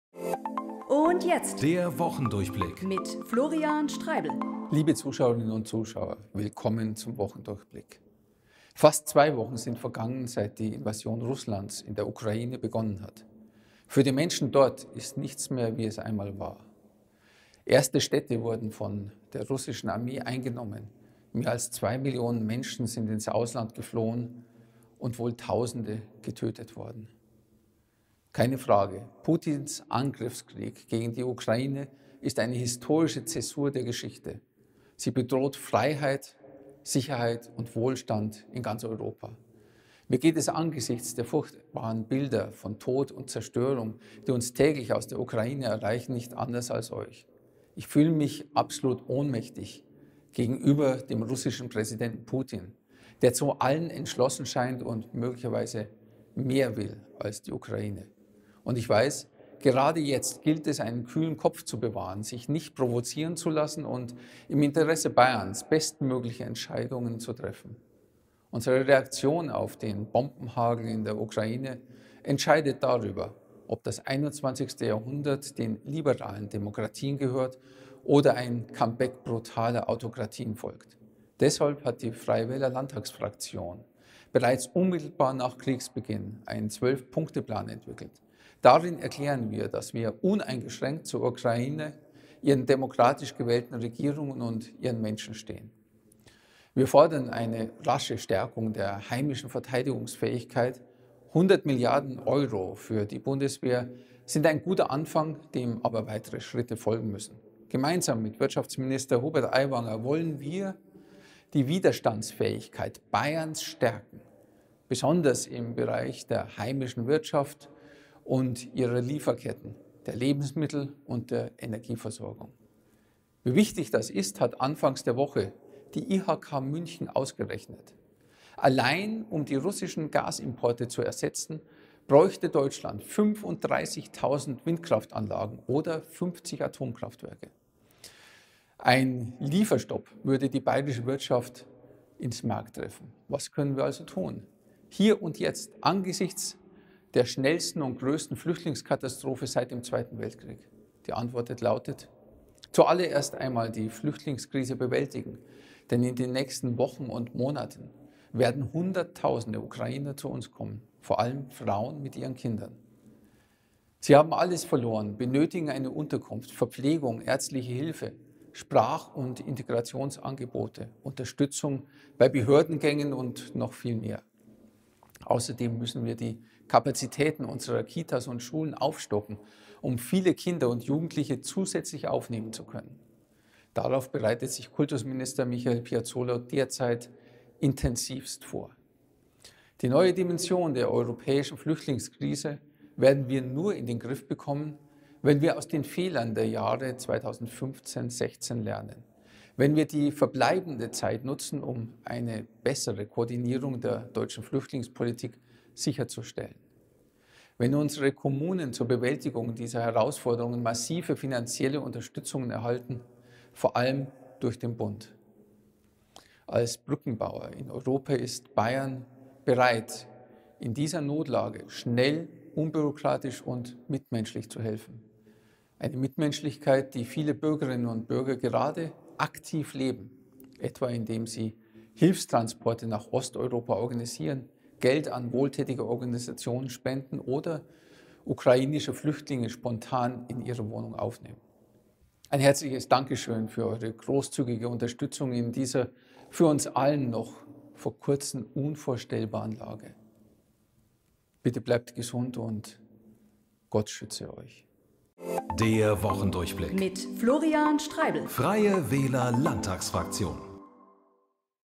Der Wochendurchblick mit Florian Streibl 09.03.2022
Krieg in der Ukraine: Warum wir jetzt einen kühlen Kopf bewahren sollten, wie wir unsere Widerstandskraft in Bayern ausbauen wollen und warum wir jetzt als erstes die sich anbahnende Flüchtlingskrise lösen müssen, sagt euch Florian Streibl.